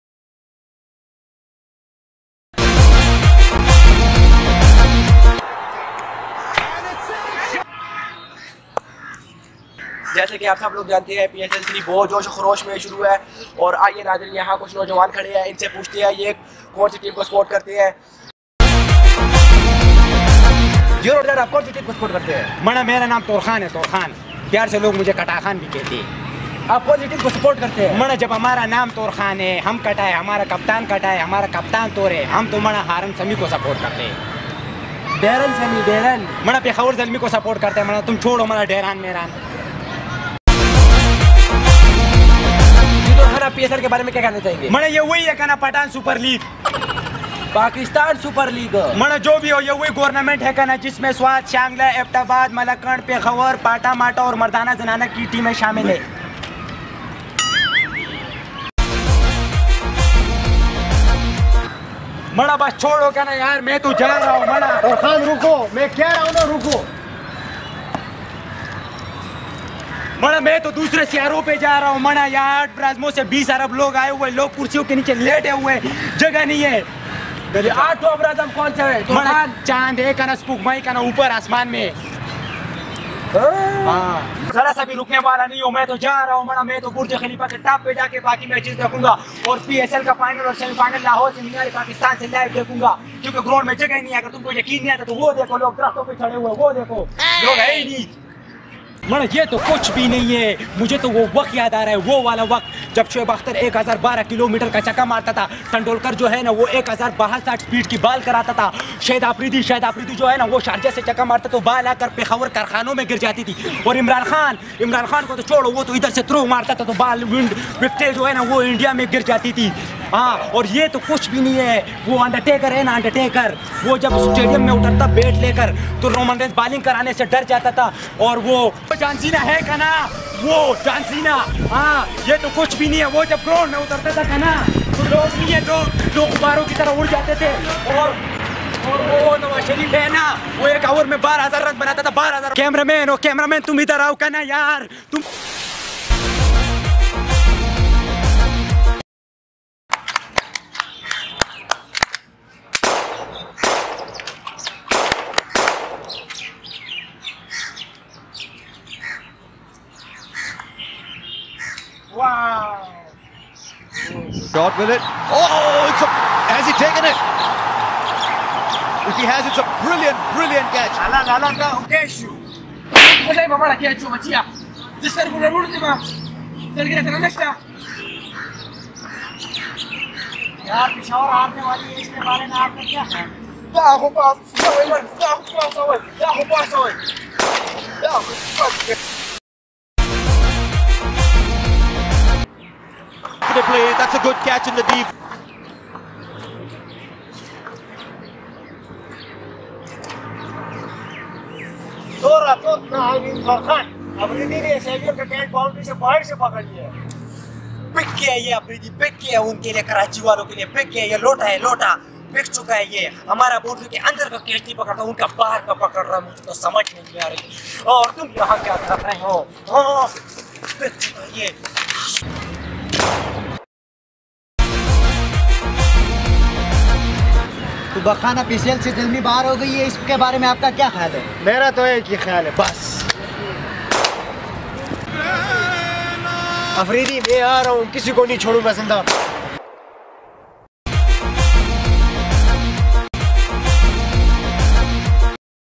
Fever Of PSL 3 Part.1 By Khan Logz Vines 2018
This Video Is About PSL 3 And In This Video We Support Peshawar Zalmi.
Khan Logz Vines